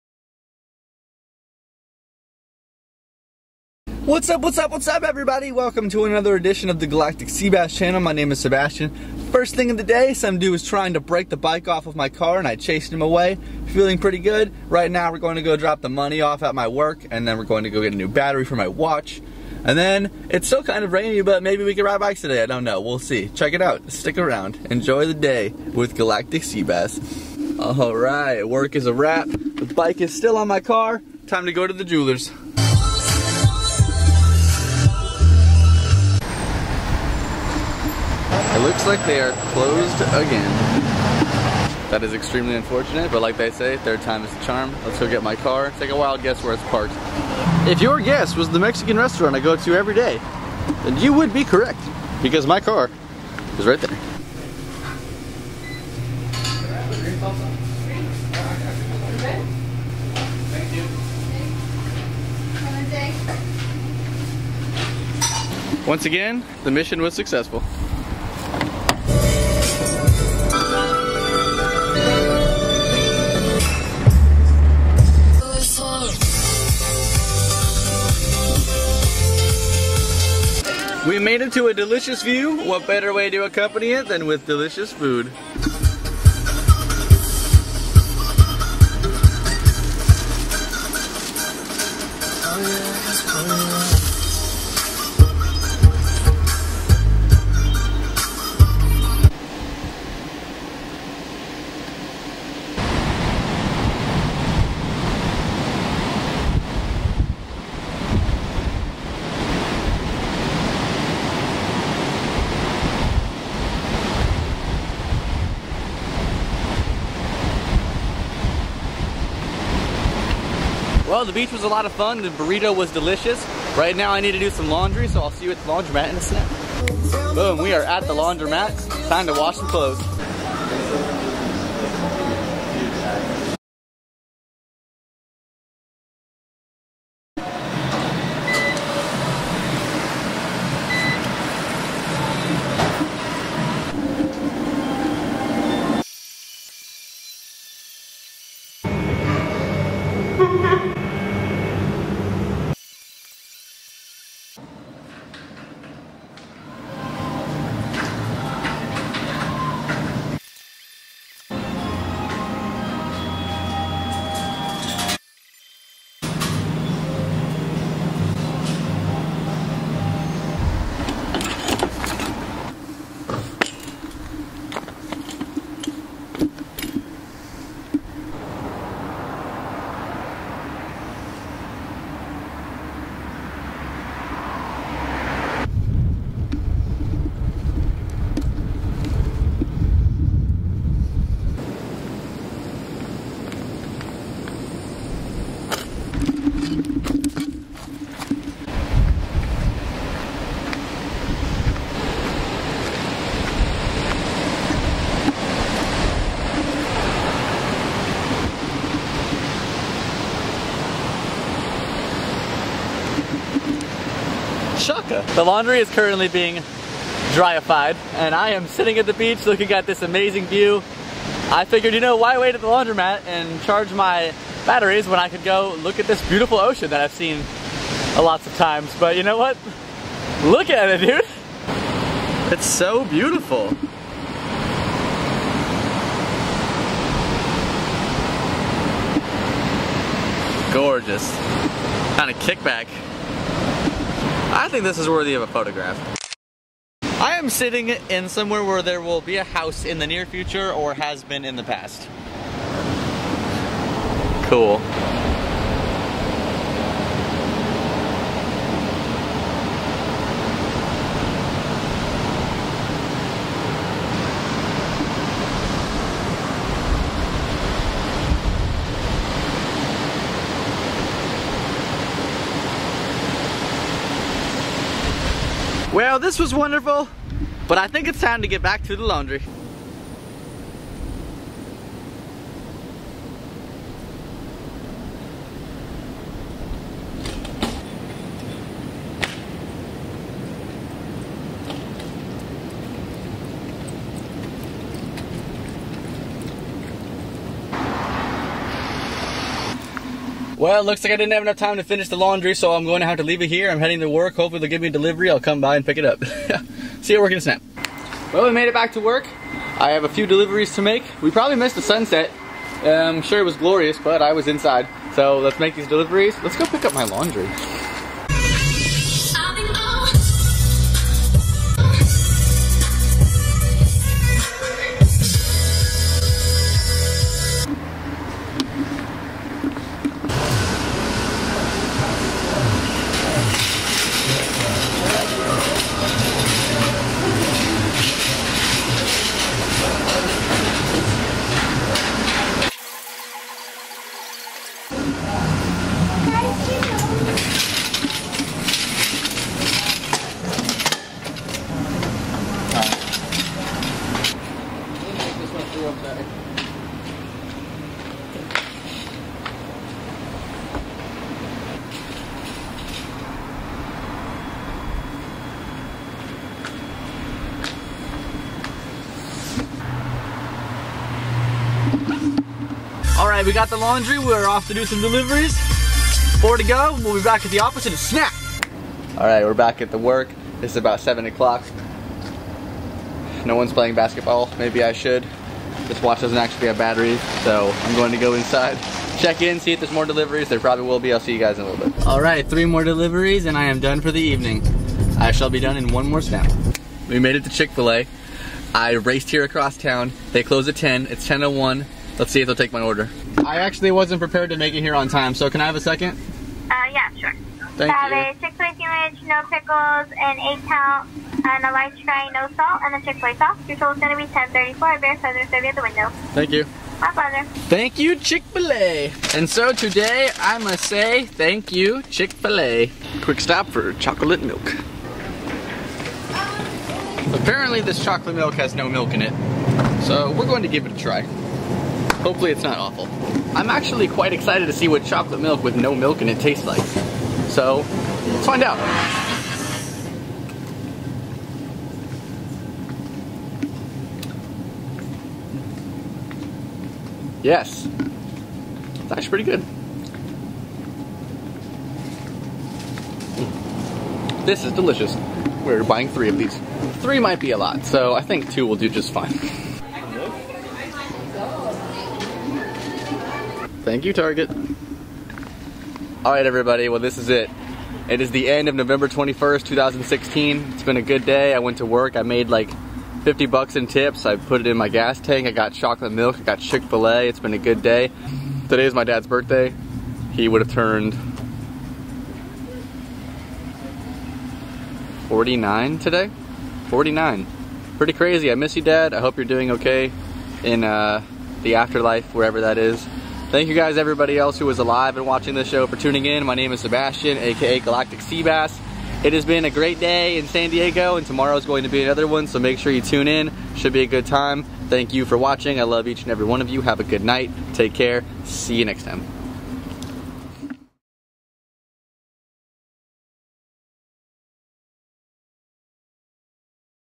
FENCES ARE THERE FOR A REASON - TO JUMP THEM
While doing some laundry by the beach, I search for the perfect view.